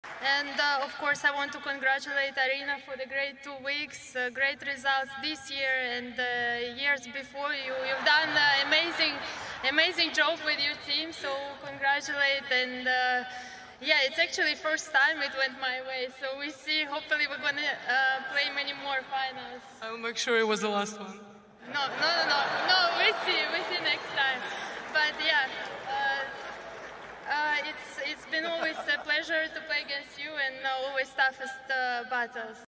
"I'll make sure it was the last one!" 🤣 Love this between Aryna Sabalenka & Elena Rybakina!
Fighting talk after the 2023 Indian Wells final between the reigning Australian Open and Wimbledon champions 😏